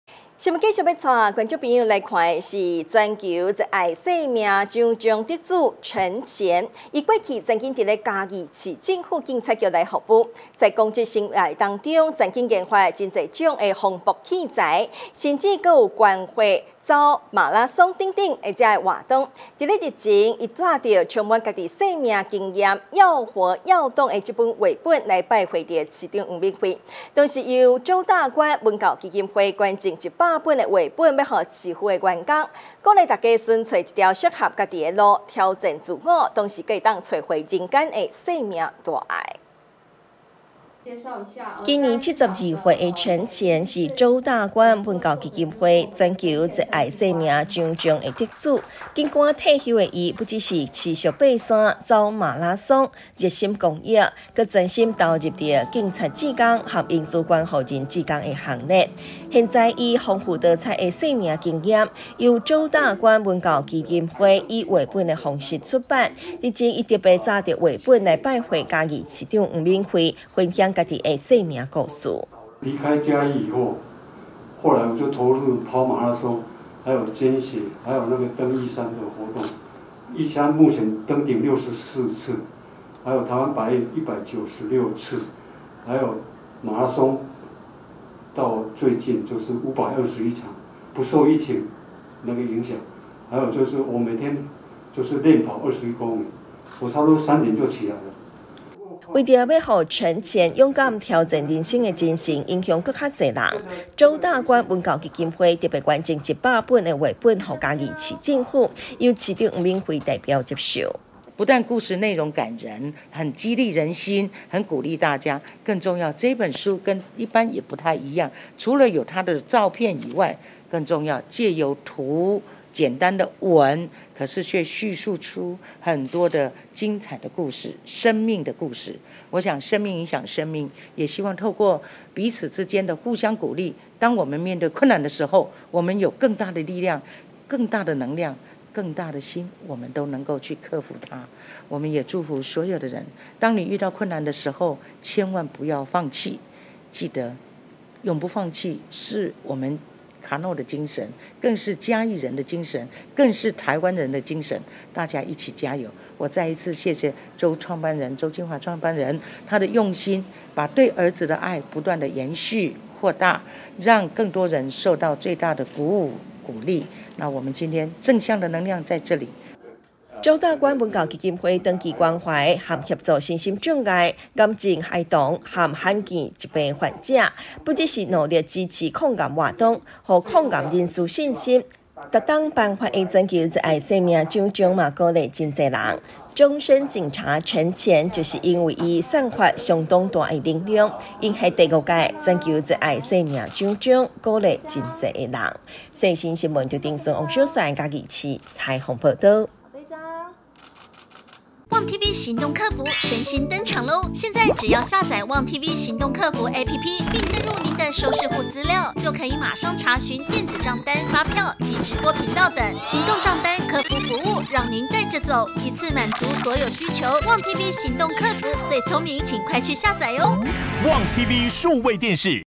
世新新聞 要活要動！終身警察陳前活出自己的奇蹟
109年8月29日 (六) 世新新聞

要活要動！終身警察陳前活出自己的奇蹟

「全球熱愛生命獎章」得主陳前，過去曾經在嘉義市政府警察局服務，公職生涯中曾研發多種防爆器材、甚至捐百血、跑百馬，日前他帶著充滿自己生命經歷的《要活要動》繪本，拜會市長黃敏惠，同時由周大觀文教基金會捐贈100本繪本給市府員工，鼓勵大家尋找一條適合自己的路，挑戰自我，找回人間的生命大愛 。